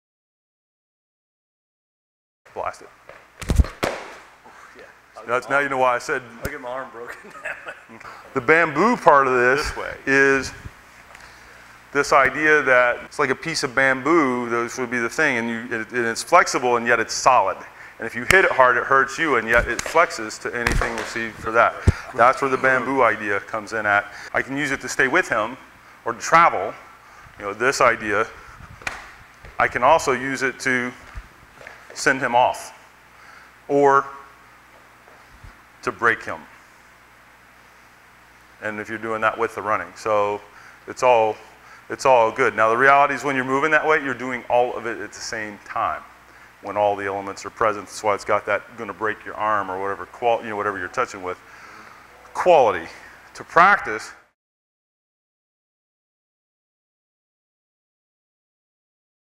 Xing Yi - Bamboo Art
(this is just one of the many internal principles and methods in Clear's Xing Yi Quan.)

Bend like a piece of bamboo.

You give way only a little while at the same time loading a powerful spring that adds a lot of power.

Relaxation is required,

...but the body is not soft.

To someone striking you it feels hard like a piece of bamboo.

Be careful.

It's very easy to injure your partner while training this method.